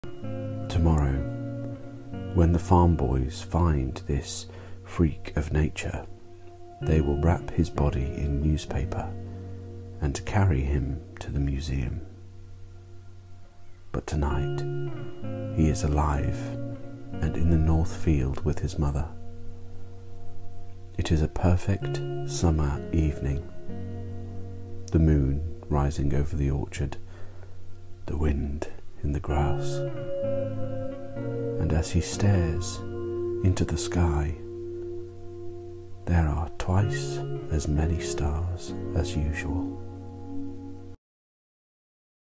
The Two Headed Calf by Laura Gilpin's [Art by Minxei]
Did this for an IG reel

Pls tell me who did the voice in this :( I can't find it since I only found the audio in Tiktok